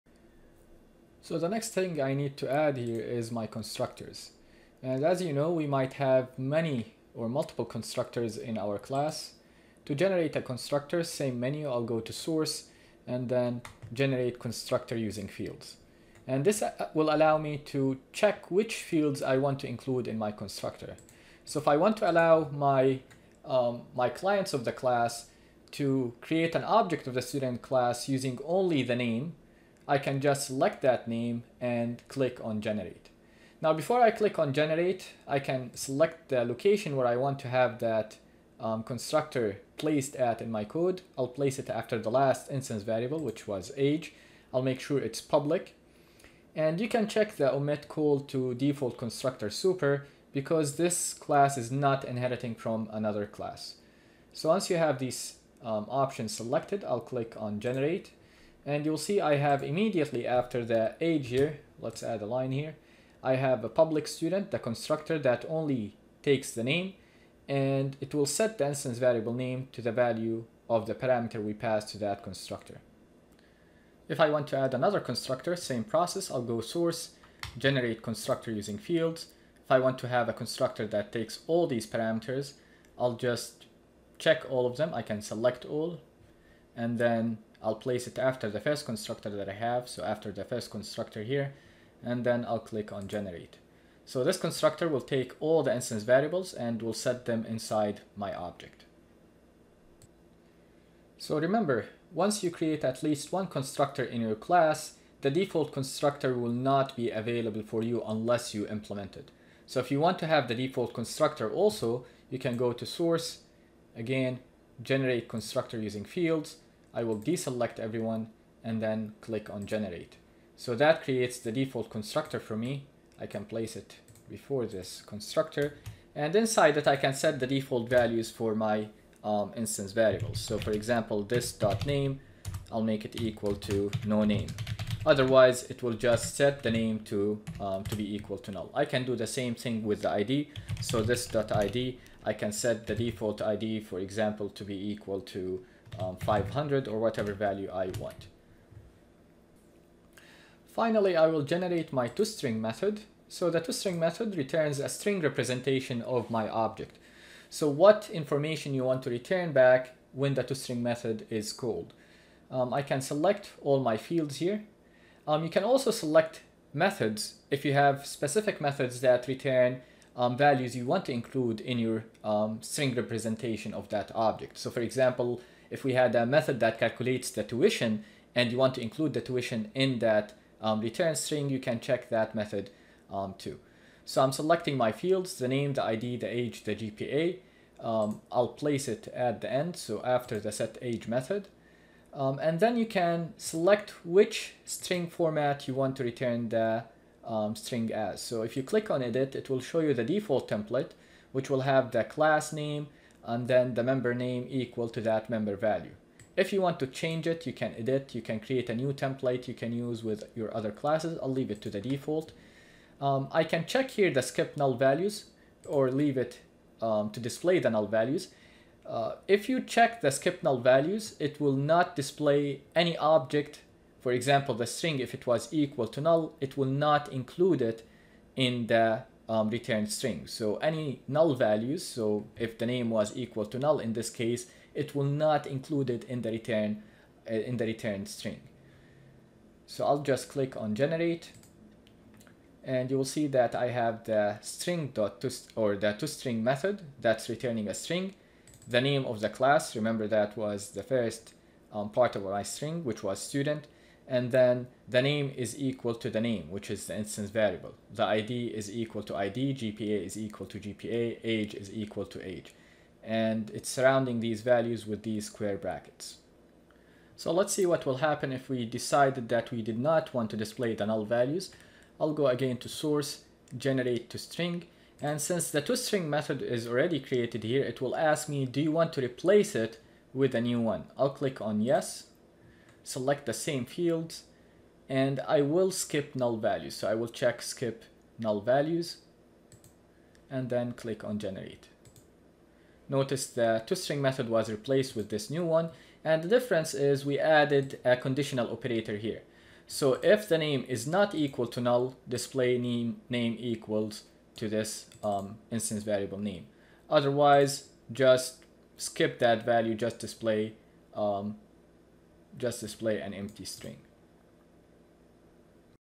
Fast Way To Create Getters, Setters, Constructors and The toString Method In Eclipse - Part2
In this video we will go over how to easily generate constructors and the tostring method for a Java class in Eclipse.